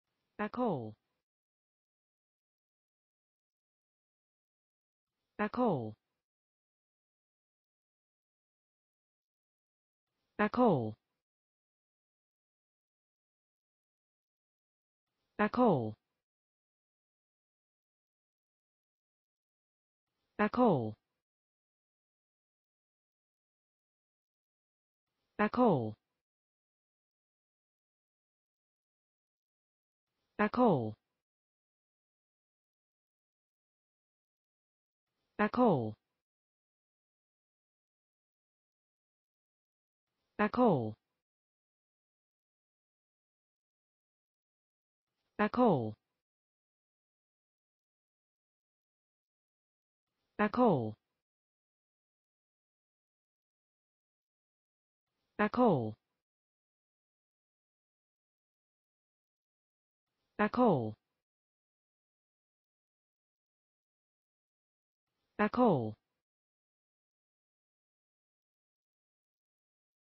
Acoel: How to pronounce Acoel in english (correct!).Start with A. Learn from me.
Follow me!Learn from me how to pronounce the word Acoel in english.This video shows you how to pronounce the word Acoel in english.

Acoel